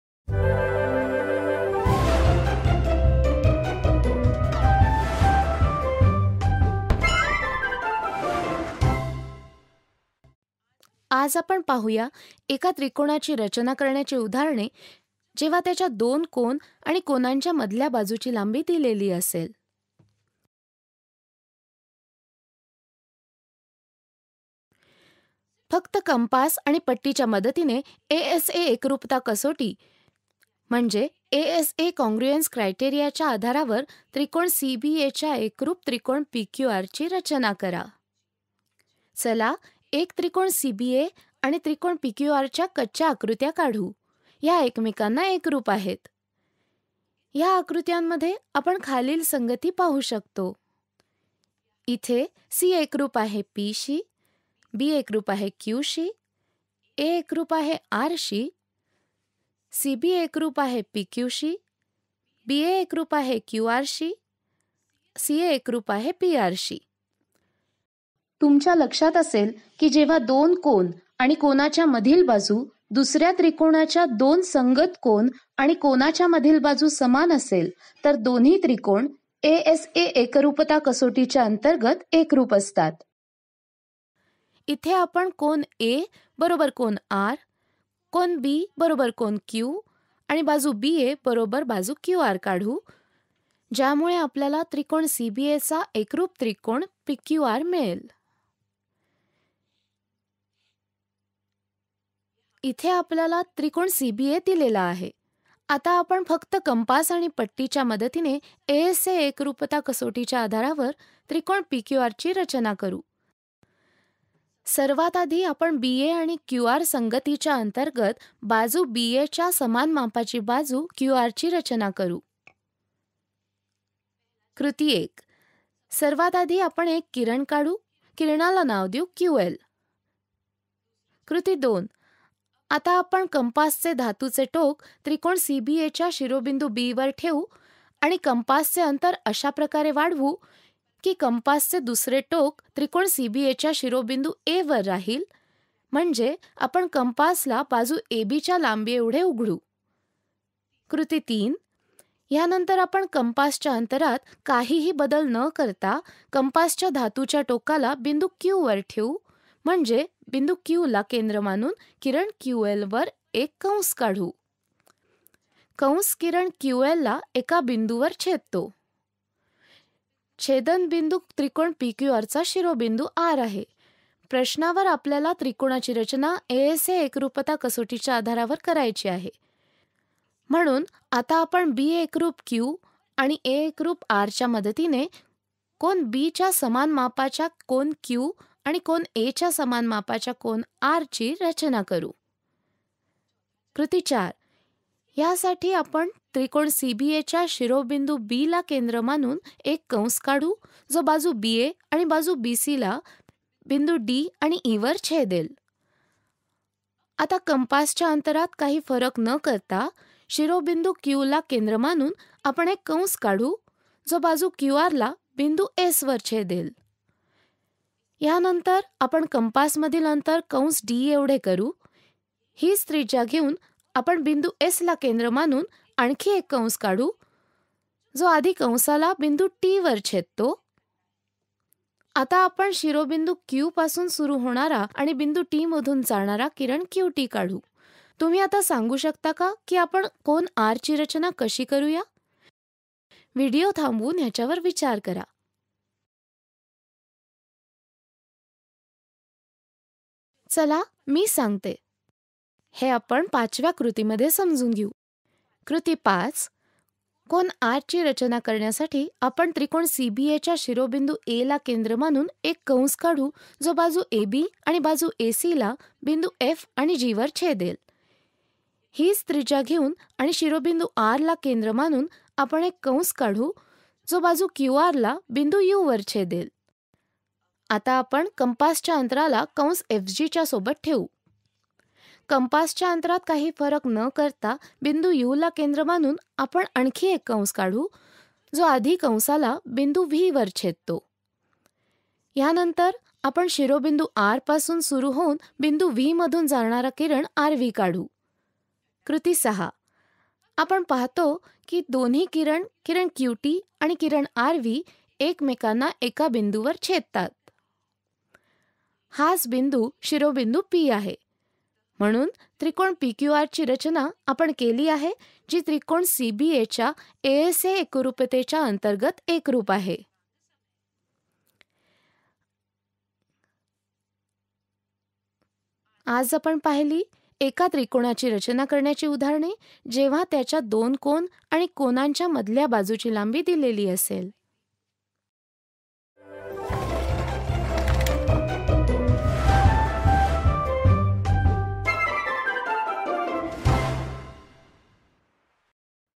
त्रिकोणाची रचना (ASA कसोटी) |Part 3/3|Construction of a triangle (ASA criterion)|Marathi|Class 7
त्रिकोणाची रचना (ASA कसोटी) |Part 3/3|Construction of a triangle (ASA criterion)|Marathi|Class 7 | प्रायोगिक भूमिती | कक्षा 7 | NCERT | प्रायोगिक भुमिती

Construction of a triangle based on ASA criterion | Practical geometry | Grade 7 | NCERT | Practical geometry

Welcome to part 3 of Construction of a triangle based on ASA criterion. In this video, we will discuss various examples of Construction of a triangle based on ASA criterion.